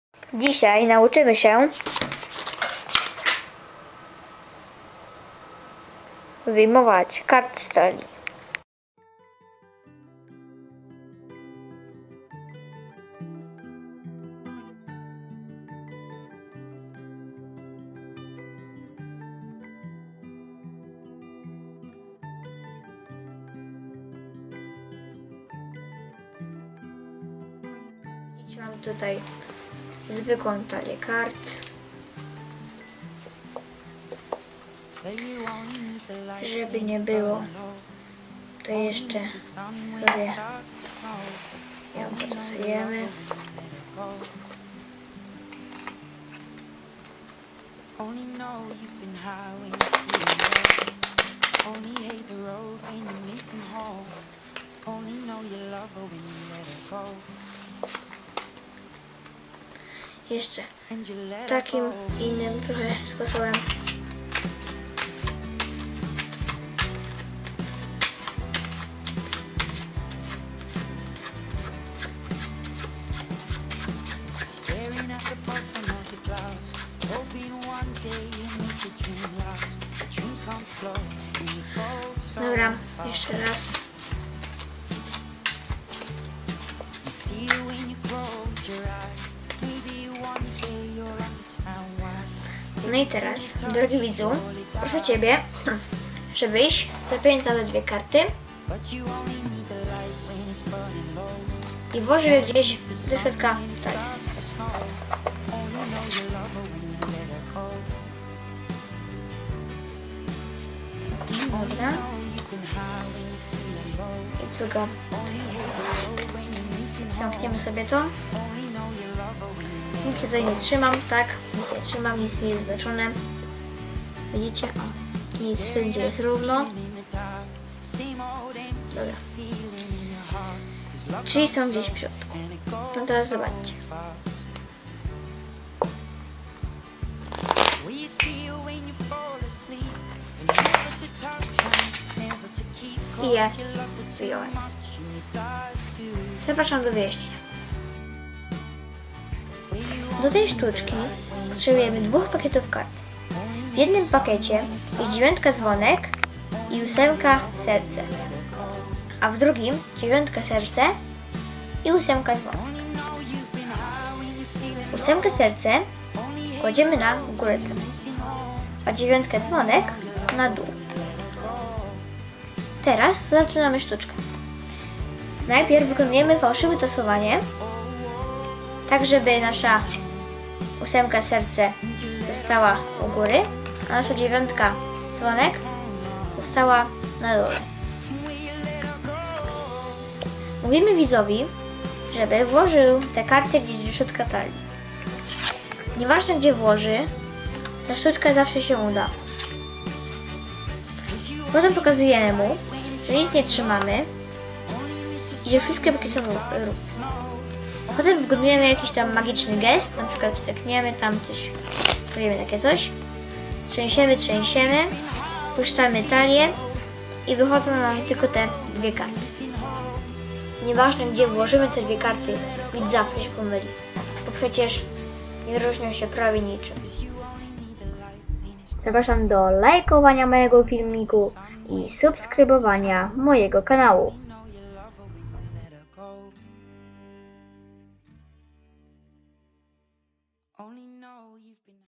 Sztuczka karciana ,,2 x czerwone'' z wyjaśnieniem
Zapraszam Was do obejrzenia mojego pierwszego filmiku z  kartami na YT. Dziś sztuczka karciana "2 x czerwone".